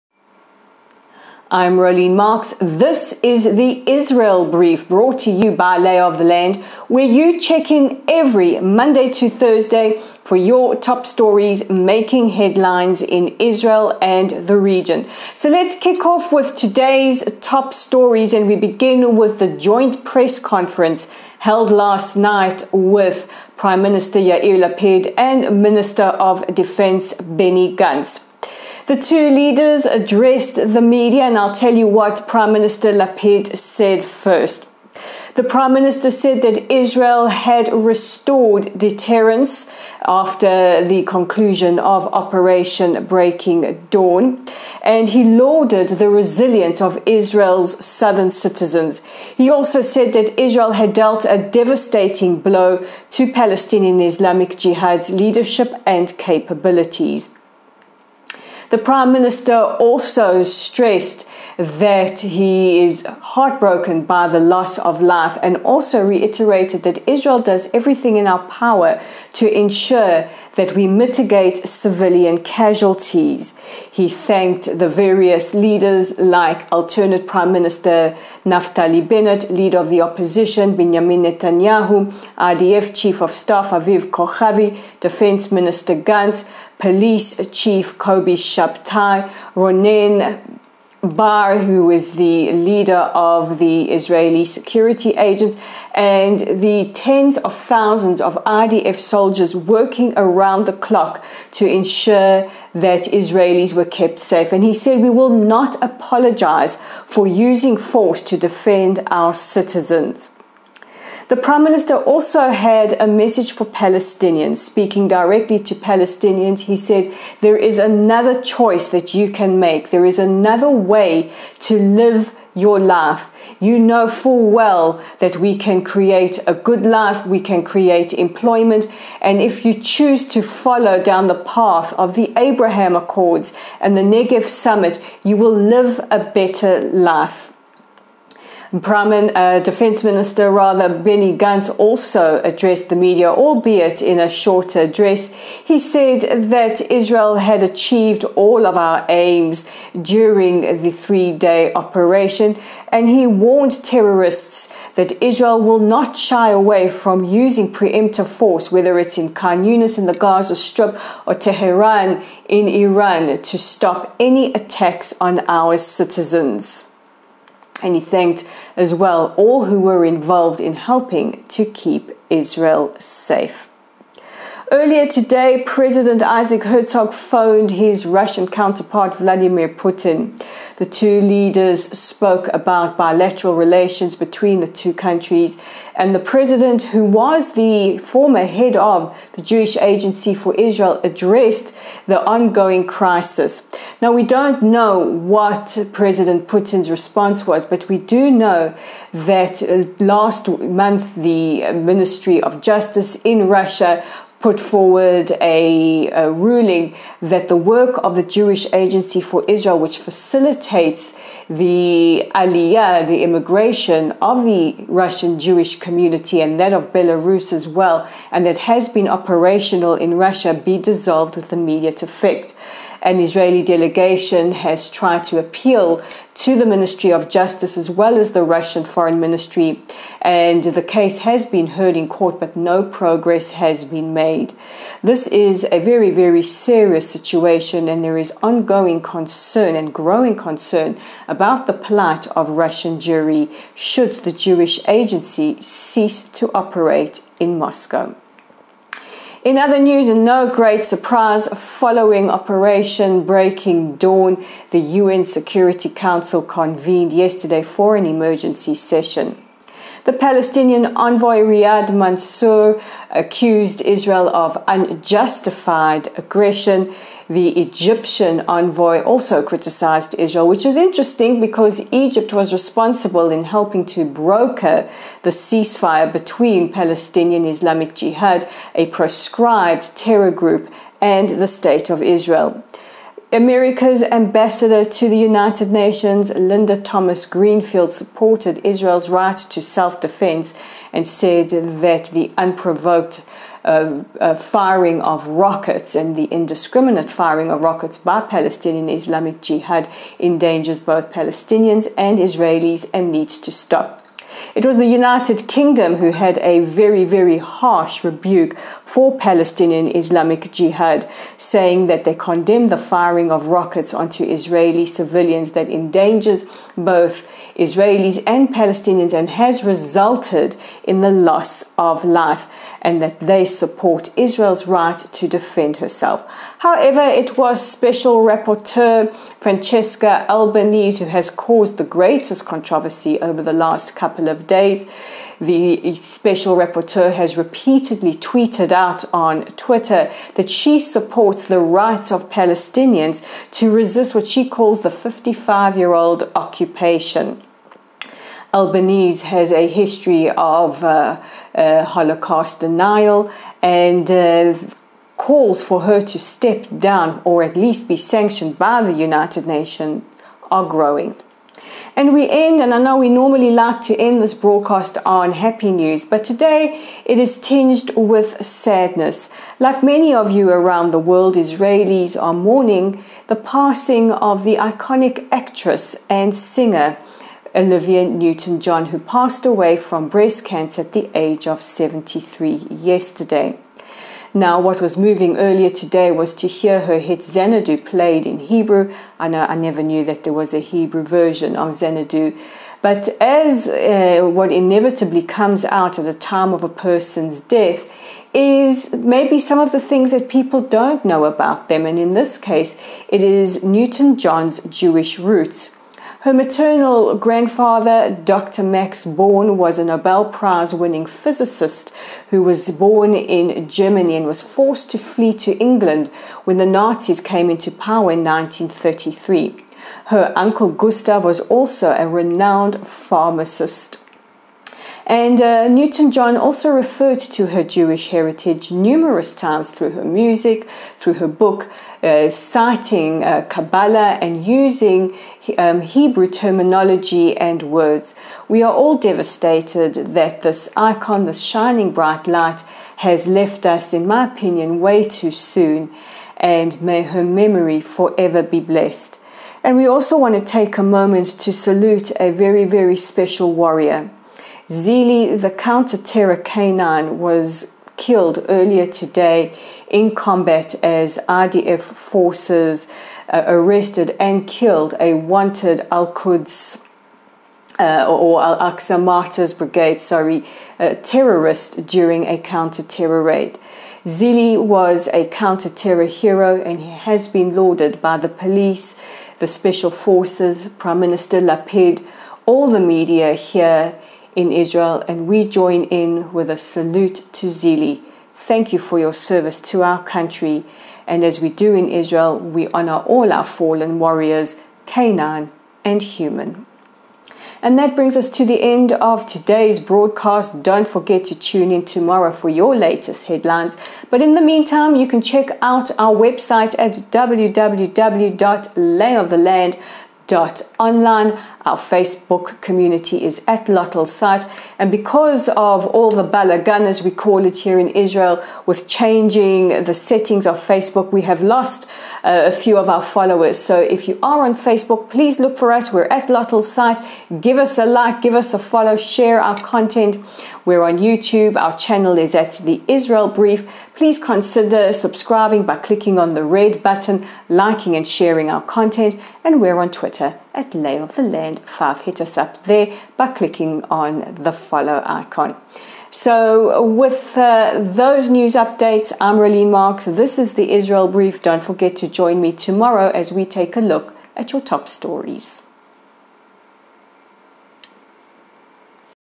The Israel Brief 9/8
PM Lapid and DM Gantz address media following Operation Breaking Dawn. Pres. Herzog speaks to Putin. UN Security Council convenes session on Israel and PIJ. Remembering Olivia Newton John and Zili, the counterterror canine.